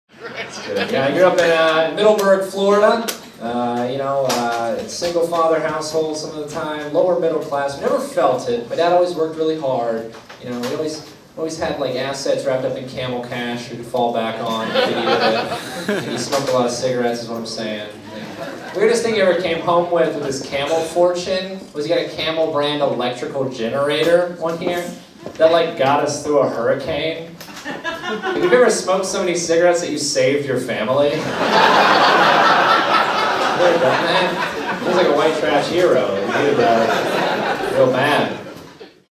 Jake Head - Camel Cash
Jake Head reminisces about his father's Camel Cash. Epic Comedy Hour January 2015